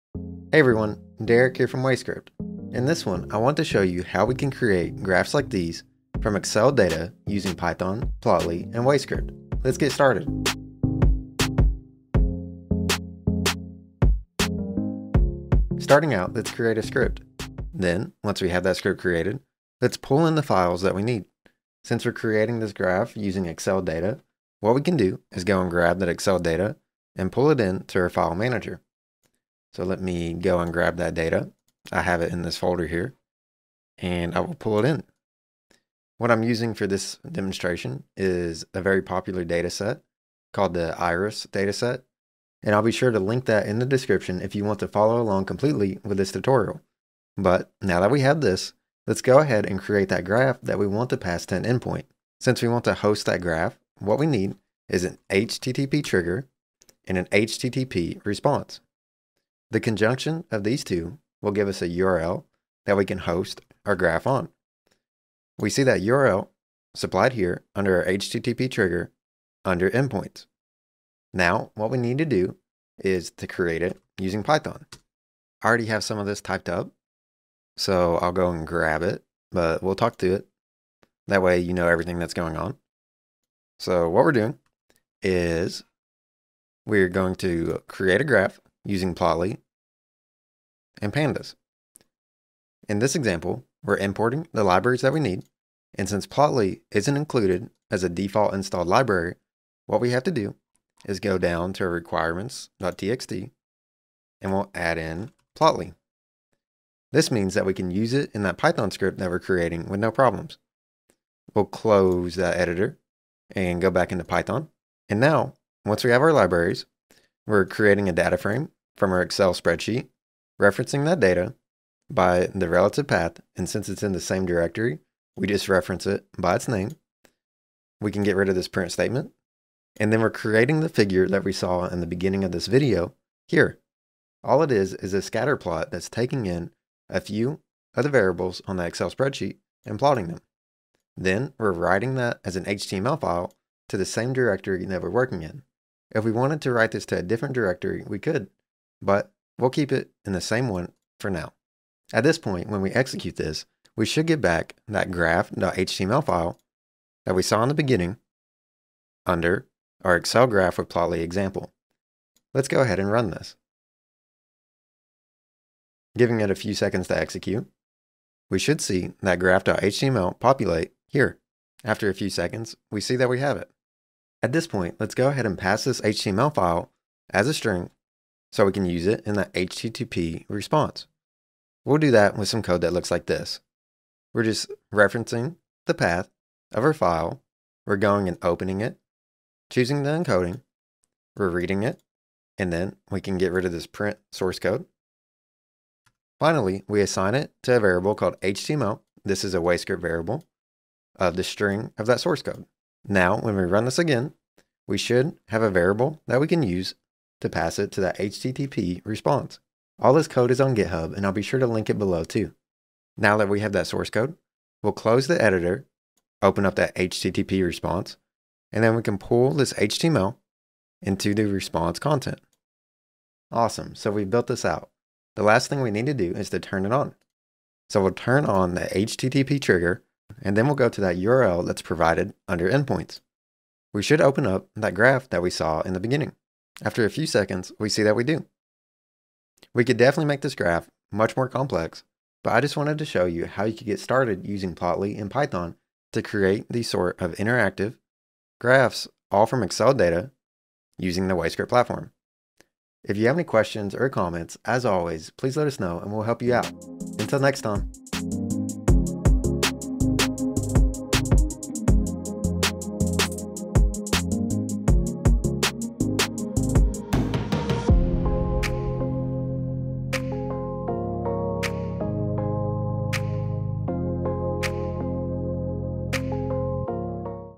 Build Hosted Interactive Graphs from Excel Data with Python, Plotly, and WayScript
*** Want to clone this script? ***

Questions about this script or anything else? We're always around to help.